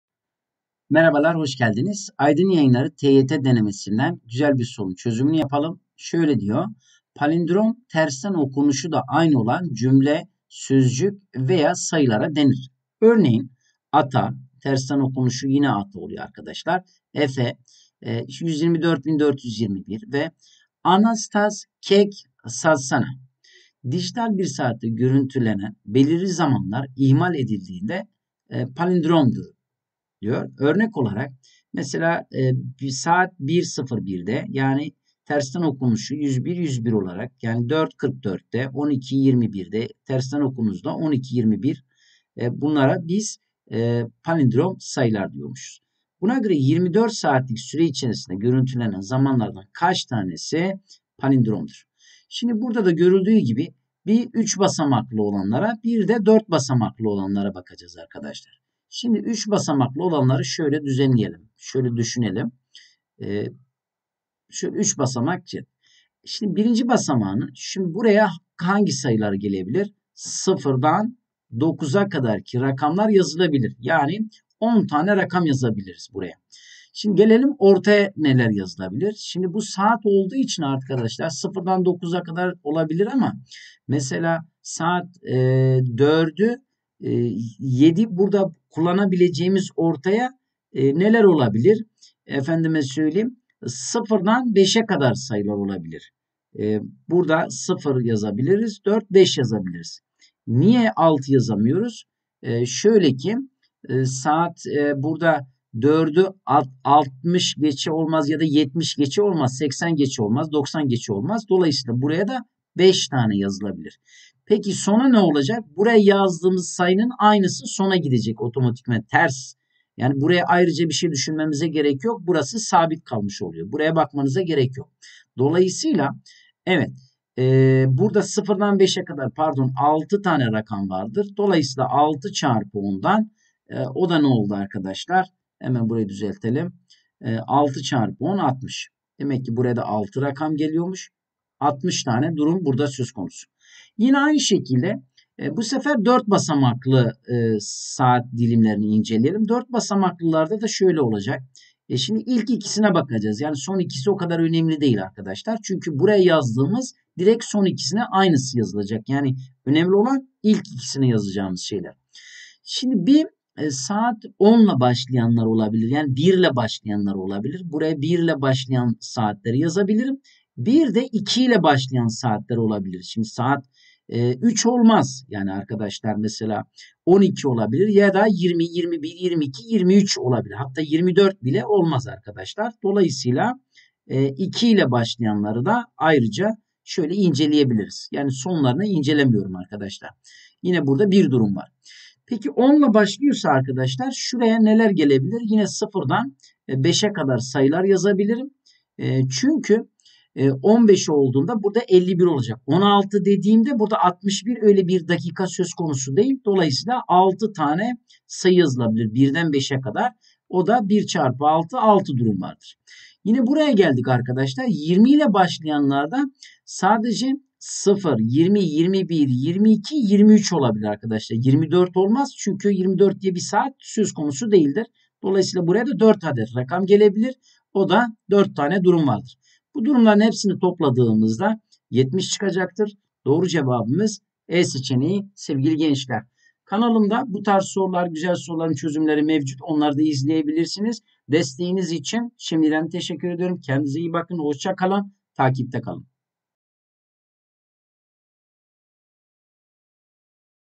PALİNDROM | AYDIN YAYINLARI | TYT DENEME SINAVI |2022
Palindrom: Tersten okunuşu aynı.
En uzun cümle ' EY EDİP ADANADA PİDE YE '

En eğlenceli ve pratik konu anlatımı videoları...

Diğer videolara aşağıdaki linkten ulaşabilirsiniz.